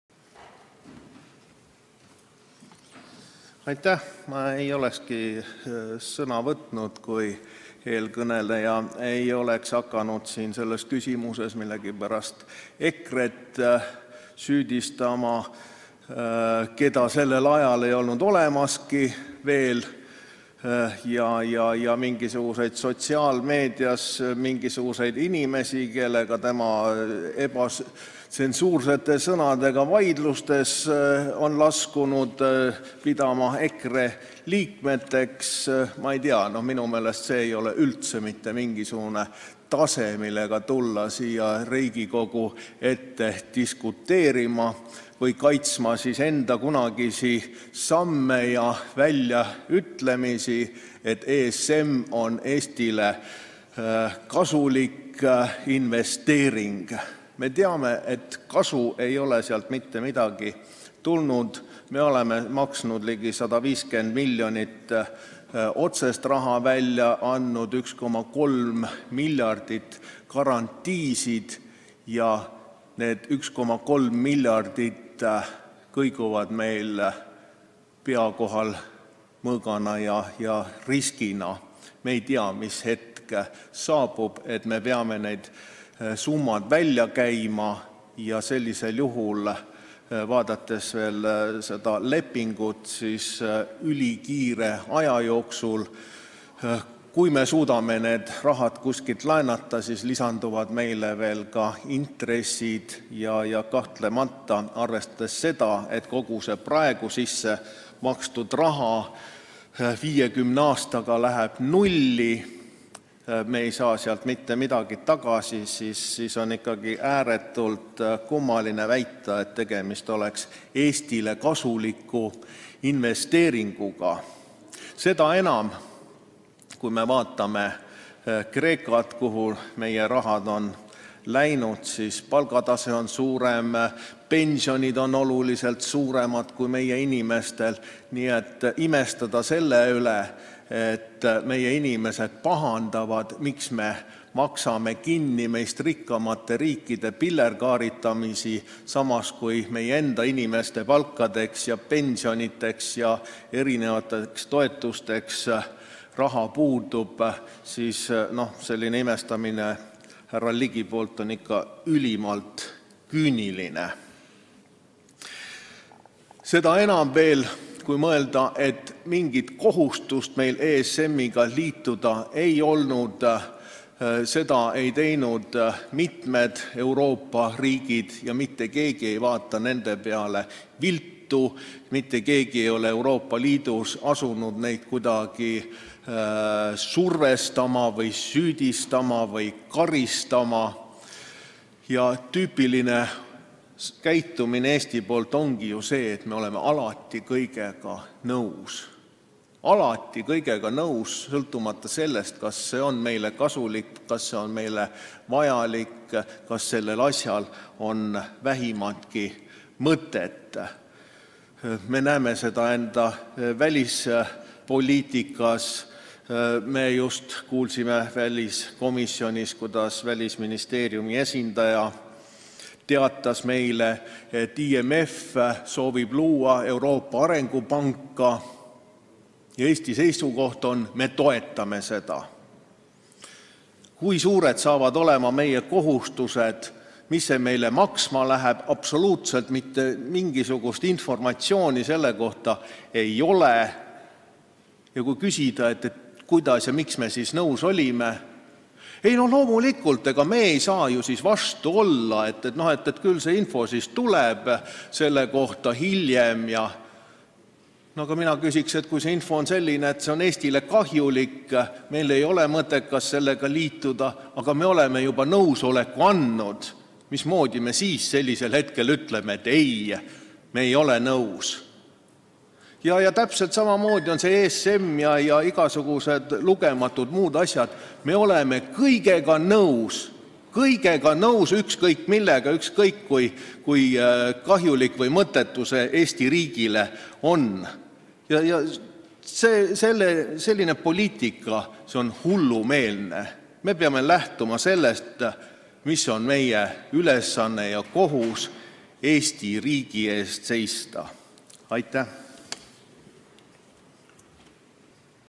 Henn Põlluaas Riigikogus: Tüüpiline Eesti käitumine on see, et me oleme alati kõigega nõus
Enam kui kolm aastat tagasi peetud kõne, mis pole oma aktuaalsust kaotanud veel tänagi, mil valitsevad taas liberaalsed erakonnad, Reformierakond ja Keskerakond.

Parafraseerides üht tuntud ütlust võib tõdeda, et küsimusi saab lahenda EKRE moodi mõistusega võttes või jürgenligilikult.

Kolmapäevasel Riigikogu istungjärgul andis rahandusminister Toomas Tõniste ülevaate Euroopa stabiilsusmehhanismi antud toetustest ja Eesti Vabariigi osalemisest selles protsessis. Kui EKRE saadik Henn Põlluaas vihjas endise rahandusministri Jürgen Ligi lubadusele, et see tuleb Eestile kasulik ja tahtis sellele tõestust, järgnes solvunud Ligi vastukäik. Et see sisaldas otseseid rünnakuid EKRE vastu, läks Henn Põlluaas veel korraks kõnepulti ja tema alljärgnev vastus on muljetavaldav.

“Ma ei olekski sõna võtnud, kui eelkõneleja [Jürgen Ligi] ei oleks hakanud selles küsimuses millegipärast EKRE-t süüdistama, keda sellel ajal ei olnud veel olemaski, ja pidama EKRE liikmeteks mingisuguseid inimesi sotsiaalmeedias, kellega tema ebatsensuursete sõnadega vaidlustesse on laskunud.

Minu meelest ei ole see üldse mitte mingisugune tase, millega tulla siia Riigikogu ette diskuteerima või kaitsma enda kunagisi samme ja väljaütlemisi, et ESM on Eestile kasulik investeering. Me teame, et mingit kasu ei ole sealt tulnud. Me oleme maksnud ligi 150 miljonit eurot otsest raha, lisaks 1,3 miljardit garantiisid ja need 1,3 miljardit kõiguvad meie pea kohal mõõga ja riskina.

Me ei tea, millal saabub hetk, kui me peame need summad välja käima, aga vaadates seda lepingut, siis ülikiire aja jooksul. Kui me suudame need rahad kuskilt laenata, siis lisanduvad meile veel ka intressid ja kahtlemata, arvestades seda, et kogu see praegu sisse makstud raha läheb 50 aastaga nulli ja me ei saa sealt mitte midagi tagasi, siis on ikkagi ääretult kummaline väita, et tegemist oleks Eestile kasuliku investeeringuga.

Seda enam, et kui me vaatame Kreekat, kuhu meie rahad on läinud, siis nende palgatase on kõrgem ja pensionid on oluliselt suuremad kui meie inimestel. Nii et imestada selle üle, et meie inimesed pahandavad selle üle, miks me maksame kinni meist rikkamate riikide pillerkaaritamisi, samas kui meie enda inimeste palkadeks, pensionideks ja erinevateks toetusteks raha ei jätku, siis selline imestamine härra Ligi poolt on ikka ülimalt küüniline.

Seda enam veel, et mingit kohustust meil ESM-iga liituda ei olnud. Seda ei teinud mitmed Euroopa riigid ja mitte keegi ei vaata nende peale viltu, mitte keegi ei ole Euroopa Liidus asunud neid kuidagi survestama, süüdistama või karistama. Ja tüüpiline käitumine Eesti poolt ongi ju see, et me oleme alati kõigega nõus, sõltumata sellest, kas see on meile kasulik, vajalik ja kas sellel asjal üldse on vähimatki mõtet.

Me näeme seda enda välispoliitikas. Me just kuulsime väliskomisjonis, kuidas Välisministeeriumi esindaja teatas meile, et IMF soovib luua Euroopa arengupanka ja Eesti seisukoht on, et me toetame seda. Kui suured saavad olema meie kohustused, mis see meile maksma läheb — absoluutselt mitte mingisugust informatsiooni selle kohta ei ole. Kui küsida, et miks me siis nõus olime, siis vastus on – ei noh, ega me ei saa ju vastu olla, küll see info tuleb selle kohta hiljem. Aga mina küsiks, et kui info on selline, et see on Eestile kahjulik ja meil ei ole mõtet sellega liituda, aga me oleme juba nõusoleku andnud, siis mismoodi me hiljem ütleme, et me ei ole tegelikult nõus?

Täpselt samamoodi on see ESM ja igasugused lugematud muud asjad. Me oleme kõigega nõus, ükskõik millega, ükskõik, kui kahjulik või mõttetu see Eesti riigile on. Selline poliitika on hullumeelne. Me peame lähtuma sellest, mis on meie ülesanne ja kohus: seista Eesti riigi eest.”

(Riigikogu istung, 21. veebruar 2018)